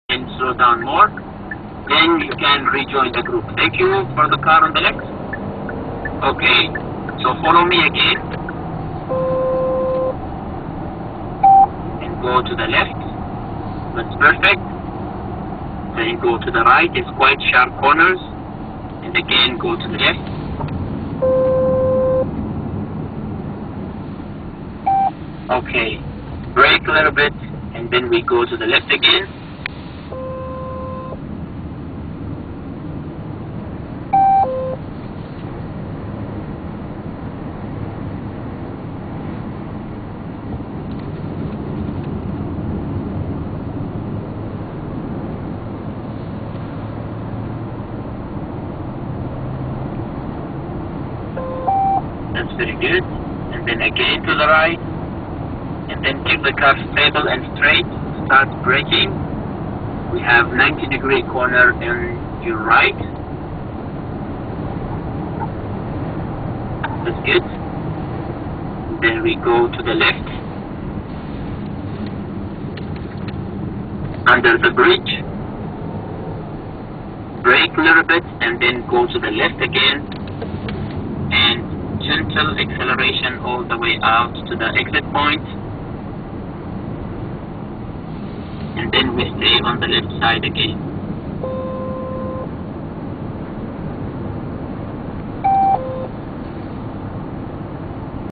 2013-2014 Hyundai Centennial launch in UAE | Yallamotor.com
First guided media drive on Yas Marina track. Read the full review on YallaMotor.com